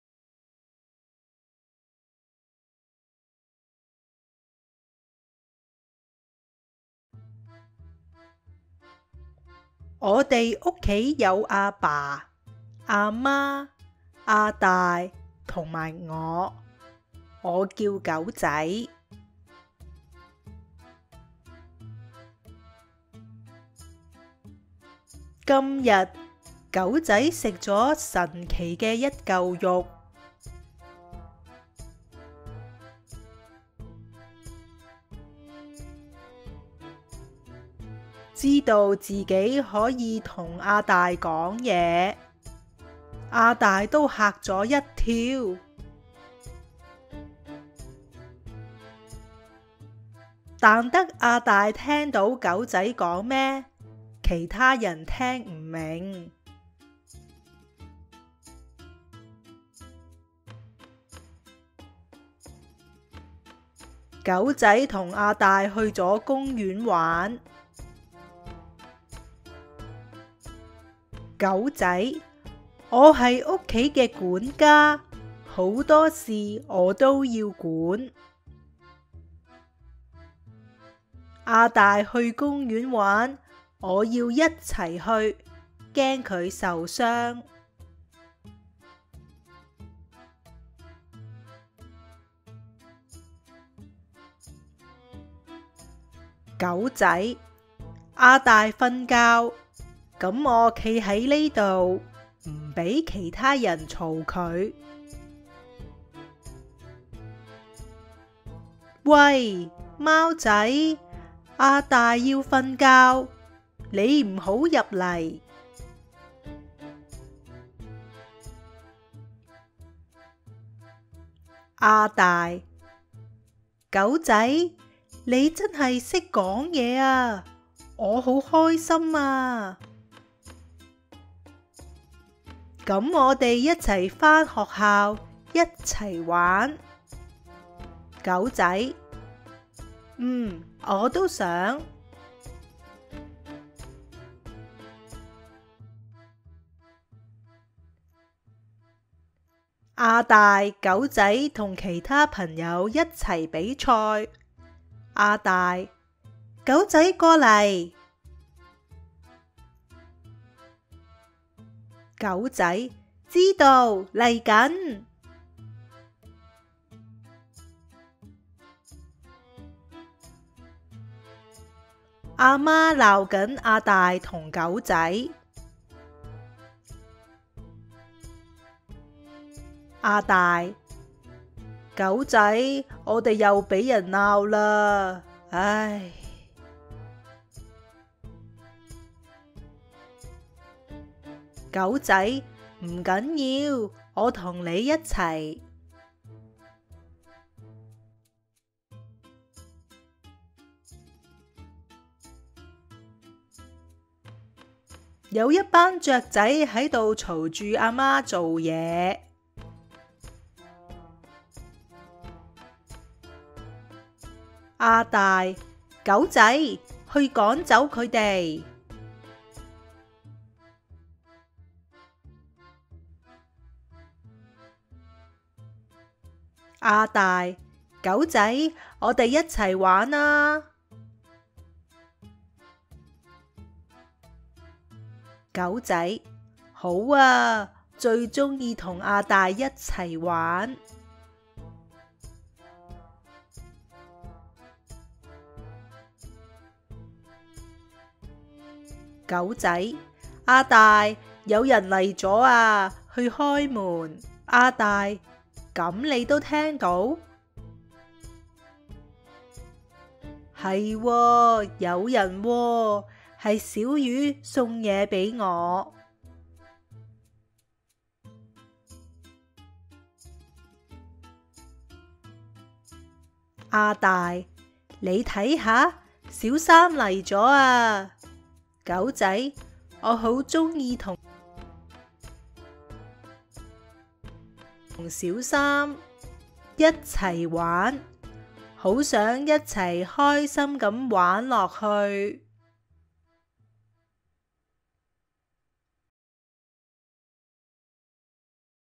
00146p:《識講嘢嘅狗仔》The Naughty Dog ---- 冚唪唥粵文讀本 HBL Cantonese Graded Readers (Lv2)
《 識講嘢嘅狗仔》係「冚唪唥粵文讀本」系列 Lv2 嘅故仔書。
"The Naughty Dog” is a title from the Hambaanglaang Cantonese Graded Readers (Lv2).

【特色 Features】
(1) 粵文書寫：用貼近廣東話口語嘅書寫系統，發揮故事嘅最佳效果。
(2) 分級詞表：本書用字按詞表編採，常用詞、緊要詞語行先，加強吸收。
(3) 粵拼：圖像化粵拼顯示。聲調用唔同顏色、角度顯示，方便記憶。

(1) Jyut-man (Written Cantonese): The text reflects spoken Cantonese. The narration of the story is directly from the script, minimising translations from the conventionally written form to the spoken form.
(2) Graded word lists: The words are selected by frequency and usage and divided into a graded scale.
(3) Jyutping romanisation: The book uses a graphical Jyutping panel with color and tone positions increasing visual anchoring By pitching the tone where you see it, confidence, engagement and internalisation occur.

【SoundCloud】 （有不同粵語口音、各種演繹方式。The same story in different accents and style.）

00:00    The Naughty Dog
00:06    Page 1
01:02    Page 2
01:33    Page 3
01:59    Page 4
02:27    Page 5
02:48    Page 6
03:22    Page 7
03:42    Page 8
04:06    Page 9
04:31    Page 10

【鳴謝 Attribution】
"The Naughty Dog" (Cantonese), translated by HamBaangLaang (© HamBaangLaang, 2022), based on the original story, Naughty Dog (English), written by Kanchan Bannerjee, illustrated by Deepa Balsavar, published by Pratham Books (© Pratham Books, 2015) under a CC BY 4.0 license on StoryWeaver.